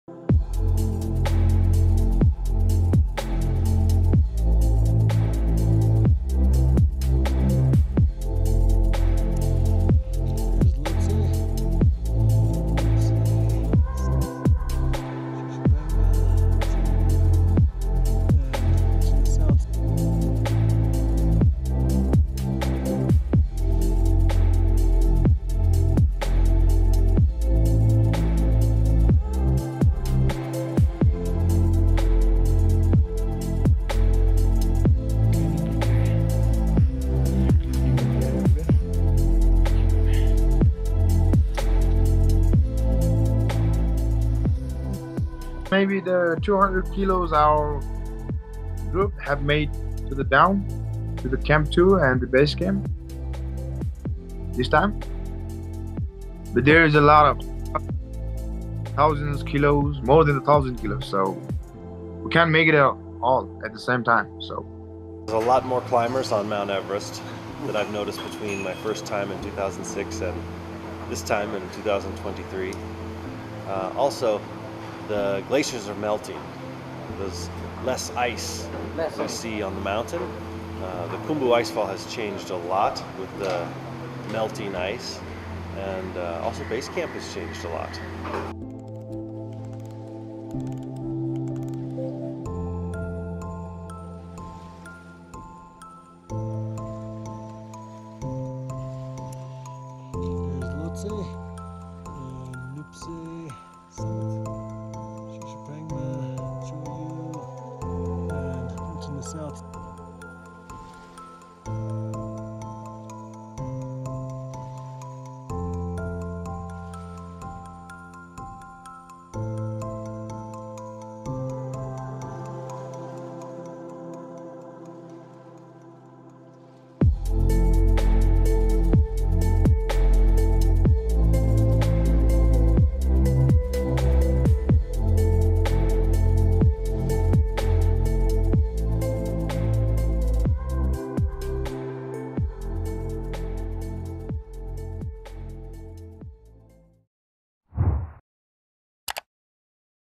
【昔日重溫】聖母峰變「全球最高垃圾場」？直擊廢棄帳篷、食物殘渣 今年已清走13噸 嚮導痛心｜Yahoo Hong Kong
聖母峰是許多登山家的終極目標之一，而隨著攀登聖母峰日益商業化，讓世界最高峰不再那麼遙不可及，但也產生許多後遺症，首當其衝的是環境問題。尼泊爾聖母峰登山嚮導丹增（Tenzi Sherpa）表示，近期已經協助清除聖母峰營地約200公斤的垃圾，估計還有上千公斤，呼籲「如果不能帶走垃圾，就不要到山上來」。他在IG上po文表示，看到山上被丟棄的帳篷，感到非常悲傷，人們因為想要減輕重量而隨意丟棄物品。
44歲的美國登山家麥迪遜（Garrett Madison）近日第13度登上聖母峰，他說與2006年相比，現在喜馬拉雅山人明顯變多，並說由於冰河融化，山上的冰也變少了。麥迪遜認為尼泊爾是「亞洲瑞士」，有潛力發展高山旅遊，但是在山上常常看到食物殘渣、食物包裝還有空氧氣瓶等，呼籲當局應該找尋更有效的辦法，監管高海拔營地。
尼泊爾當局今年已從聖母峰與附近的洛子峰清出了13噸垃圾，事實上，攀登聖母峰規定須將自己的垃圾帶下山，才能取回4000美元（約新台幣12.3萬元）的押金。登山為尼泊爾帶來可觀的收入，2023年3月到5月的登山季，當局就發放了478份許可證，創下紀錄，每份費用為1.1萬美元（33.8萬元）；而本季已有數百人嘗試登聖母峰，其中12死、5失蹤。